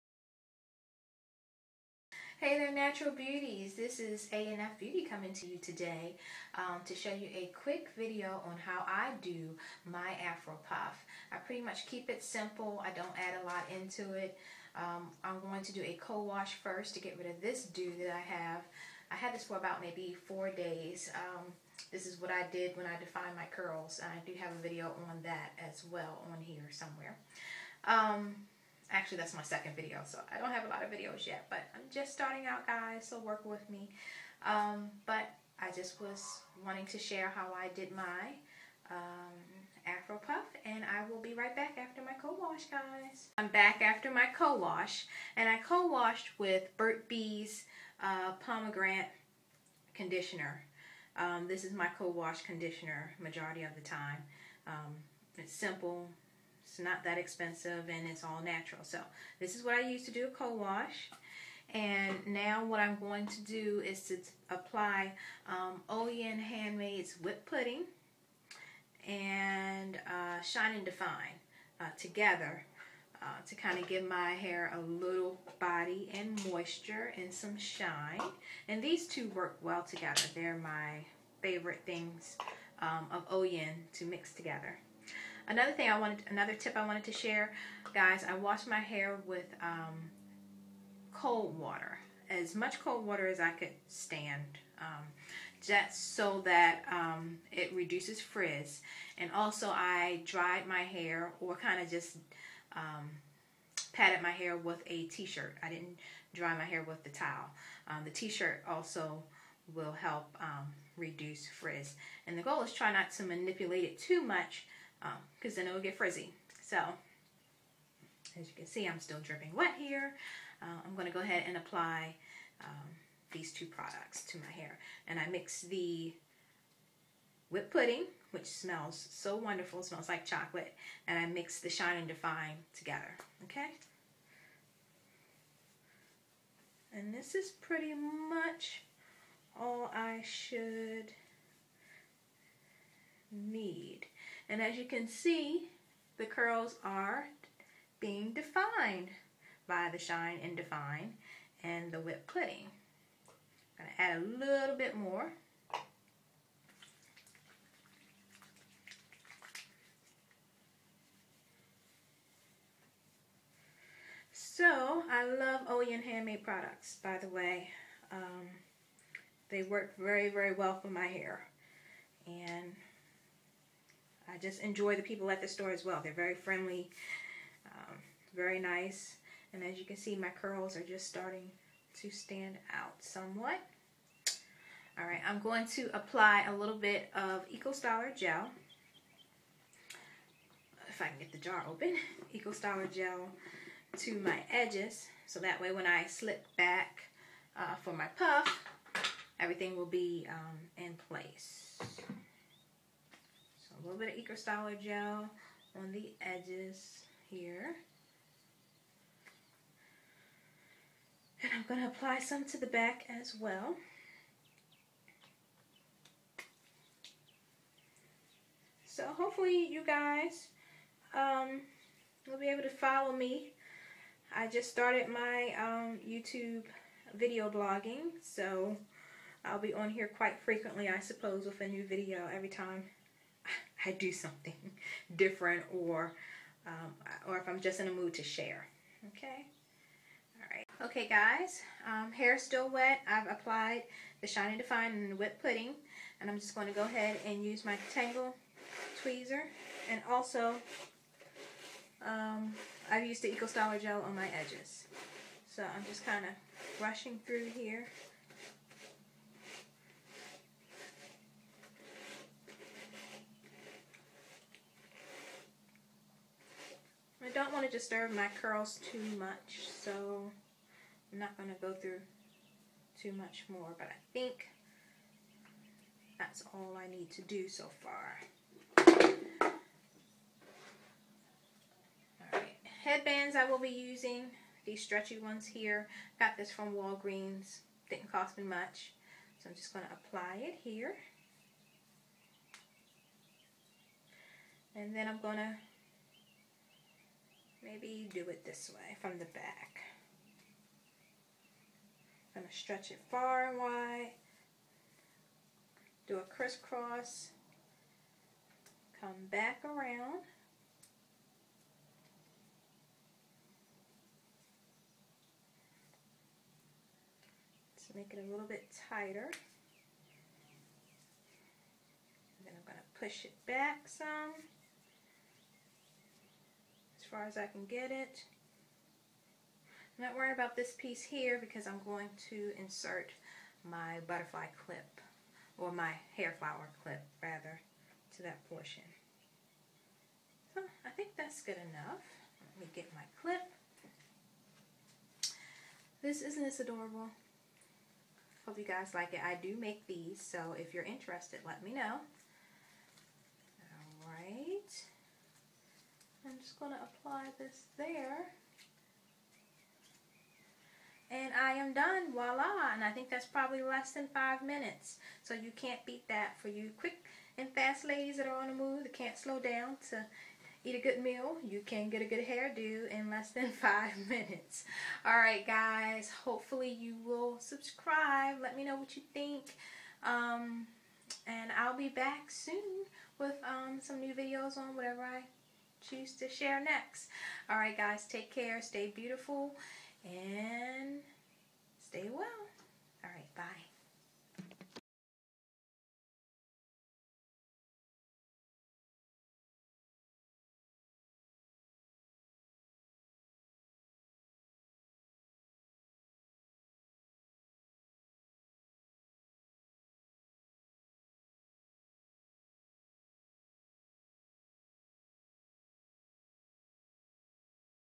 AFNBeauty TWA Afro Puff
This is a simple video on how to do a Afro puff using Oyin Handmade products and Eco Styler Gel.